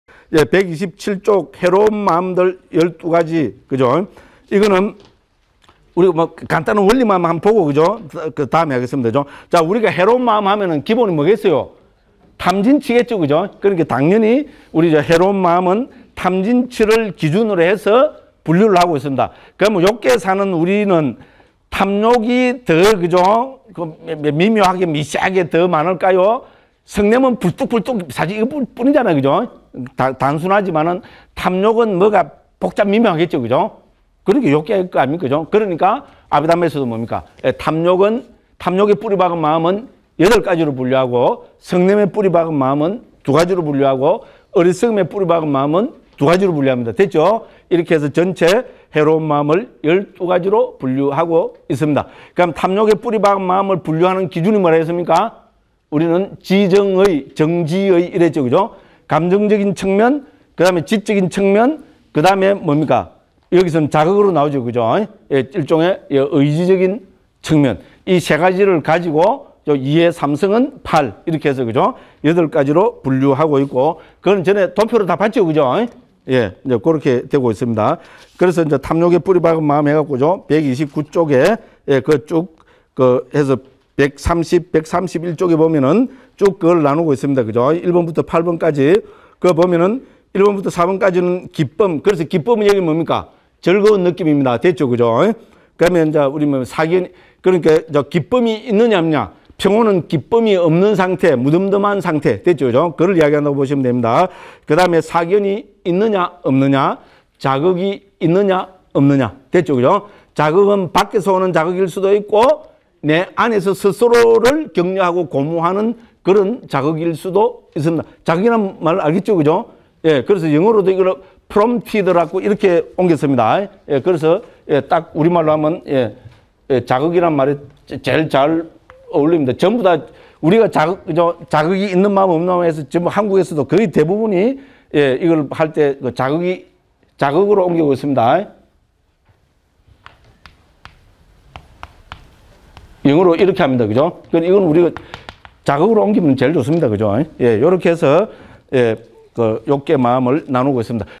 아비담마 길라잡이 18 제1권 p127 제1장 마음의 길라잡이 – 해로운 마음들 개관
2017 각묵스님 아비담마 길라잡이 (2017 전정판 1쇄 강의 )
불광사 교육원 2017년 4-2강의 中 단락 편집판 (여담제거)

전정판 1권 p127 -